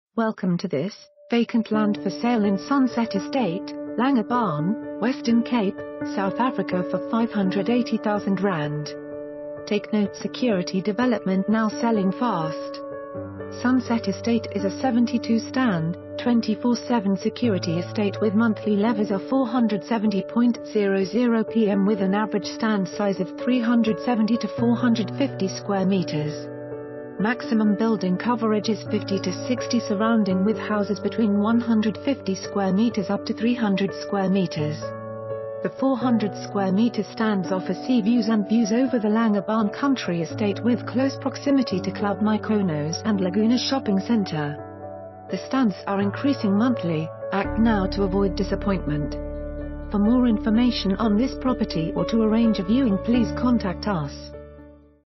Vacant Land For Sale in Sunset Estate, Langebaan, Western Cape, South Africa for ZAR 580,000
Land for sale in Sunset Estate in Langebaan!

Do not miss out! This is one of a few properties still available.

Come build your ideal holiday home in this prime secure development in Langebaan within walking distance of the beach.

Sunset Estate is situated close to Club Mykonos holiday resort, Laguna Mall and Curro Private...